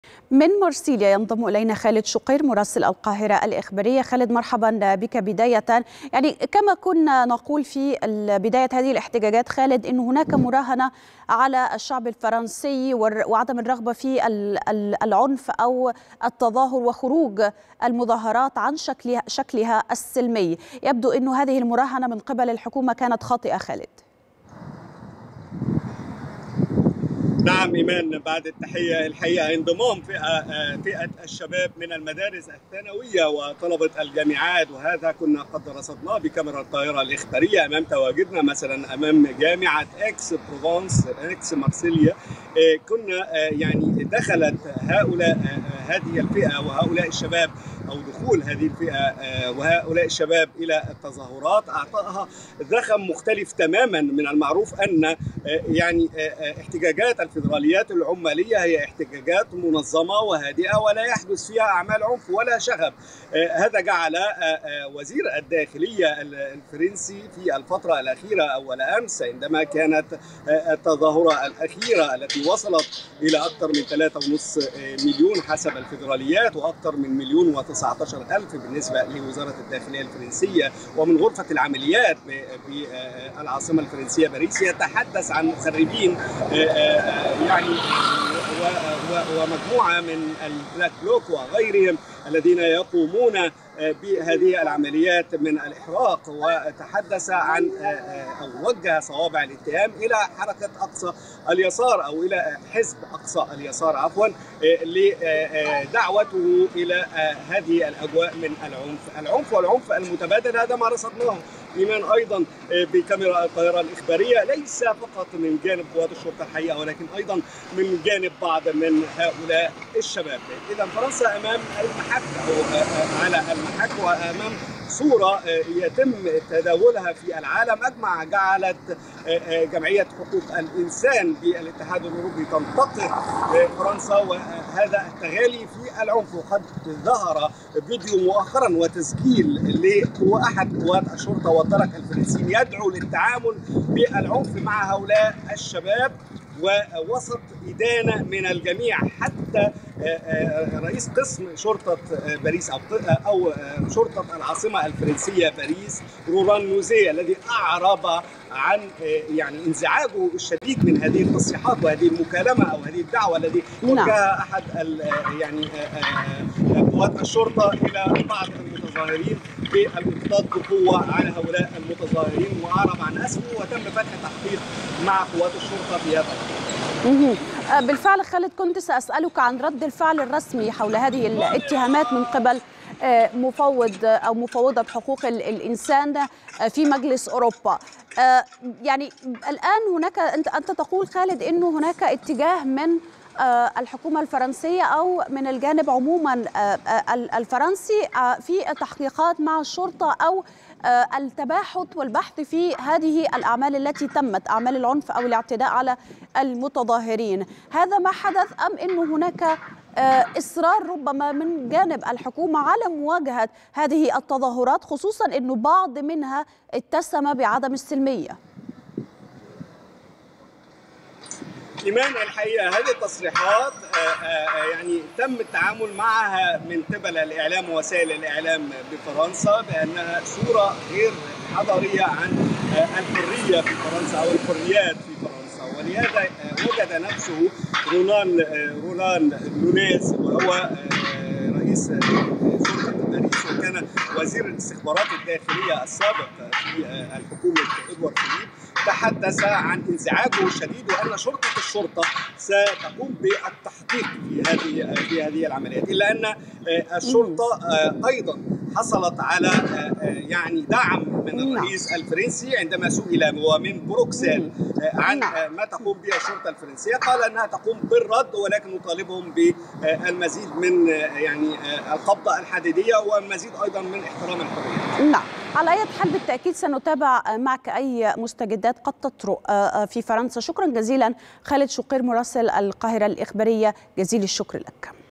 عنف في الشوارع الفرنسية.. إلى متى يستمر التصعيد ؟
عنف في الشوارع الفرنسية..  إلى متى يستمر التصعيد ؟ مراسلنا خالد شقير يُجيب

25/3/2023